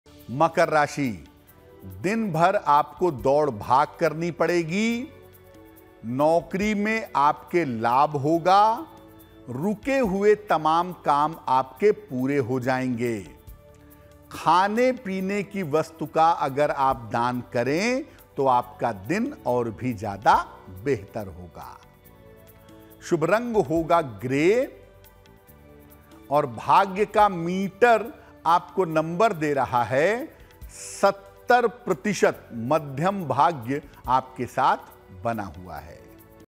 Aaj Ka Makar Rashifal 18 अप्रैल 2023: रुके हुए काम बन जाएंगे | Capricorn Horoscope Today
Aaj Ka Rashifal Makar Rashi 18 अप्रैल 2023: मकर राशि के जातकों का ज्योतिष के अनुसार रुके हुए काम बन जाएंगे. दिन भर दौड़ भाग करनी पड़ेगी. नौकरी में लाभ होगा.  खाने की वस्तु का दान करें. आज  शुभ रंग ग्रे है.